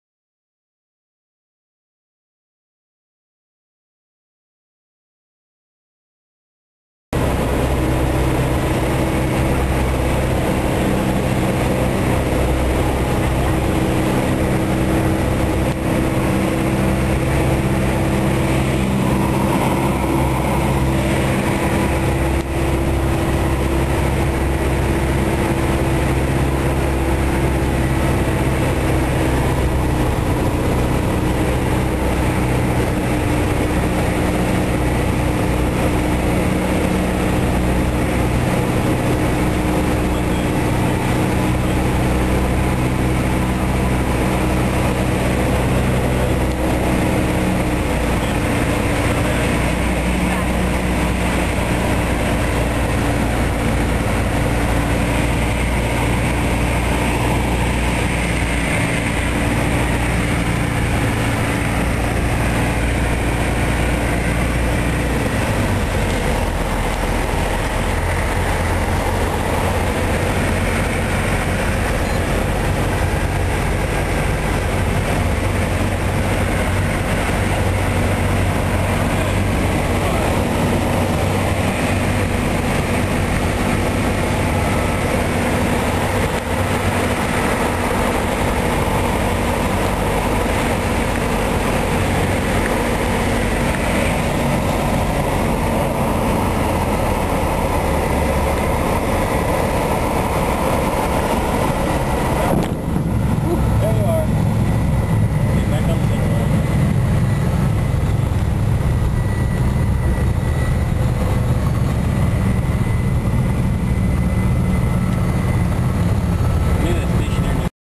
Landing in Winnemucca
Landing in Winnemucca, NV after a cross country flight that started in Caldwell, Idaho. We were going here for a 99's meeting. Yes, the landing is a bit rough...